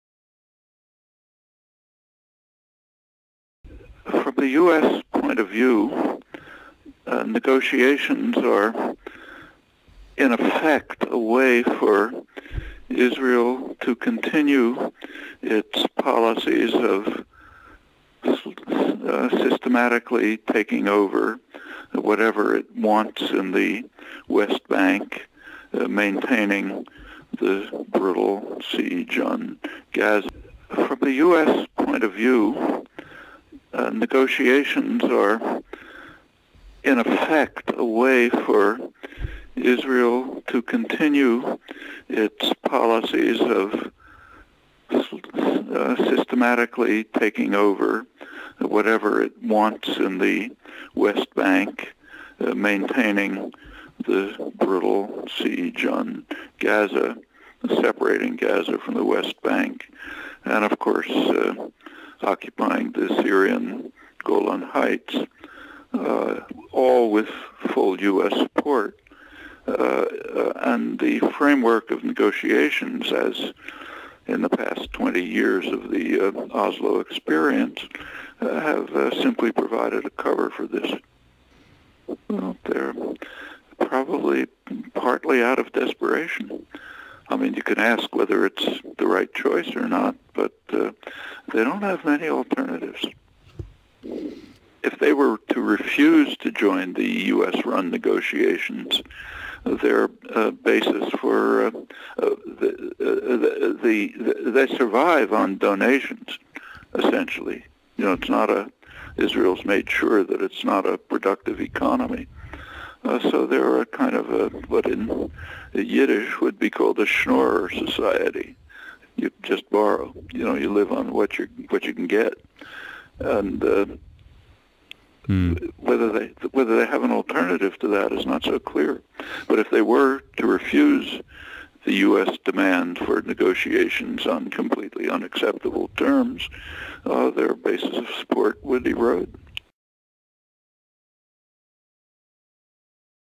Noam Chomsky on Israel Palestine Negotiations 2013
Noam Chomsky is interviewed on the ongoing Israel and Palestine "peace" negotiations, and why Palestinian leaders continue to negotiate given the Israeli goverment continues with their illegal occupation and settlements.
Edited audio originally interviewed by Frank Barat for Ceasefire.
Animation by Newsli.